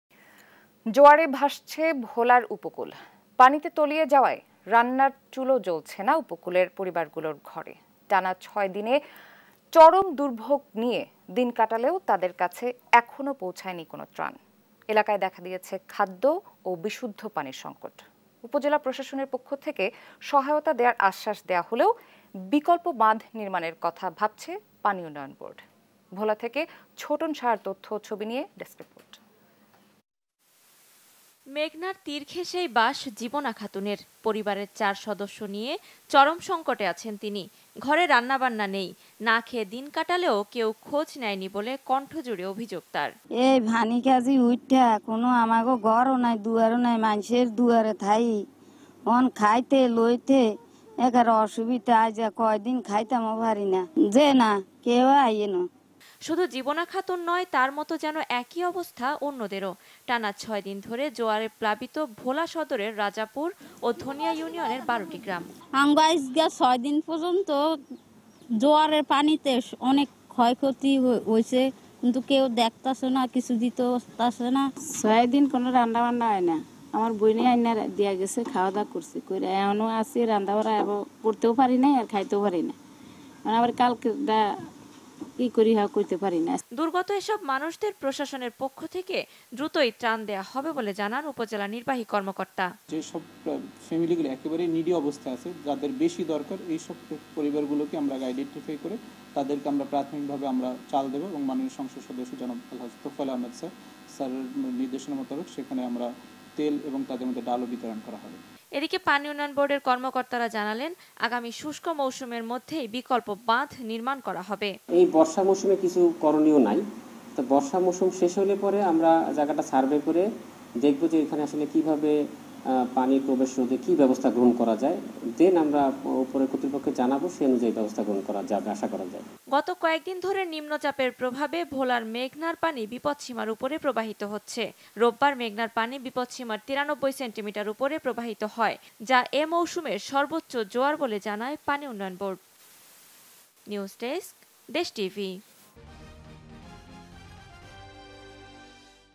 ৬ দিনে ধরে চরম দুর্ভোগ, এখনও পৌছায়নি কোন ত্রাণ | Bhola | Flood | Desh tv news
জোয়ারে ভাসছে ভোলার উপকূল।পানিতে তলিয়ে যাওয়ায় রান্নার চুলো জ্বলছে না উপকূলের পরিবারগুলোর ঘরে। টানা ৬ দিনে ধরে চরম দুর্ভোগ নিয়ে দিন কাটালেও তাদের কাছে এখনও পৌছায়নি কোন ত্রাণ। এলাকায় দেখা দিয়েছে খাদ্য ও বিশুদ্ধ পানির সংকট।উপজেলা প্রশাসনের পক্ষ থেকে সহায়তা দেয়ার আশ্বাস দেয়া হলেও বিকল্প বাঁধ নির্মাণের কথা ভাবছে পানি উন্নয়ন বোর্ড।

»» Office Address:
42, Shaheed Sangbadik Selina Parveen Sarak, Malibag 1217 Dhaka, Dhaka Division, Bangladesh